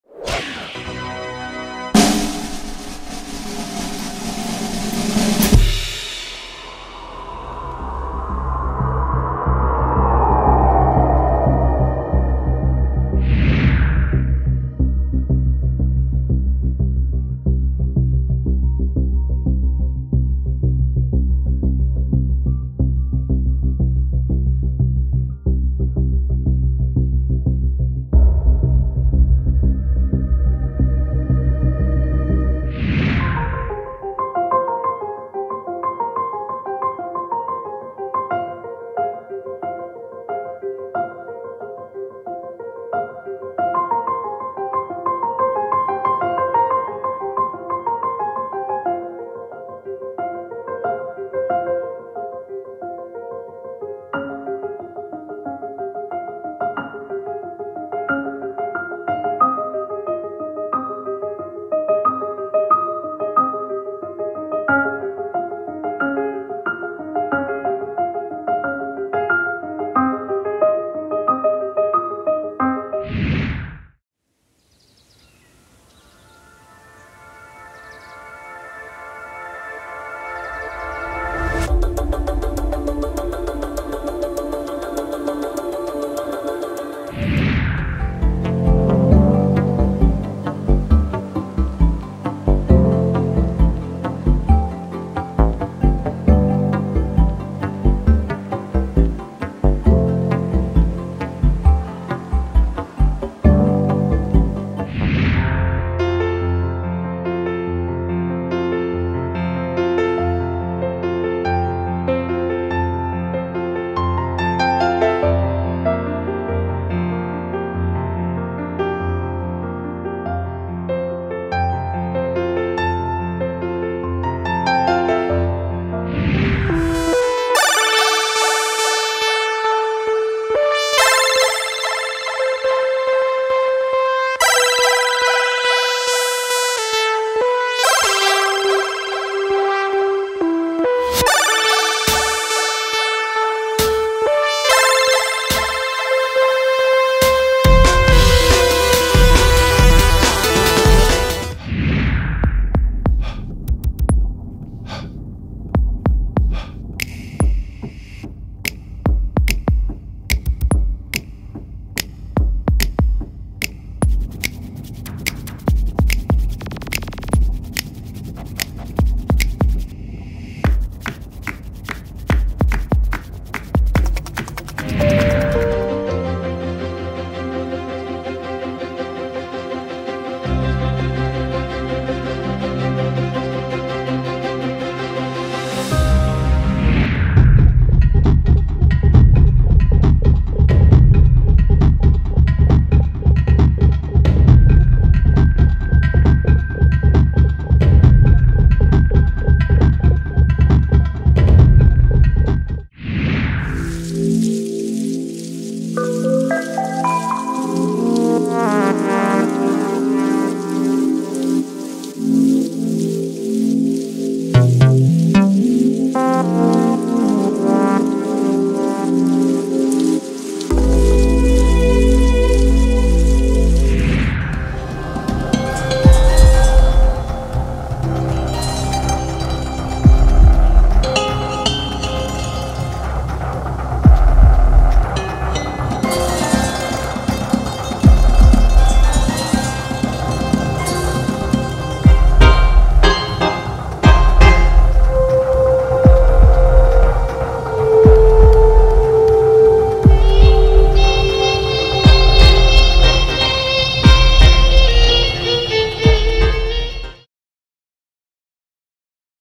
Teaser du Temps Fort Cirque 2024
Voici le teaser du Temps Fort Cirque 2024 sur le thème "ENTRE-TEMPS".

Vidéo et montage réalisé par Vyacheslav Iroshnikov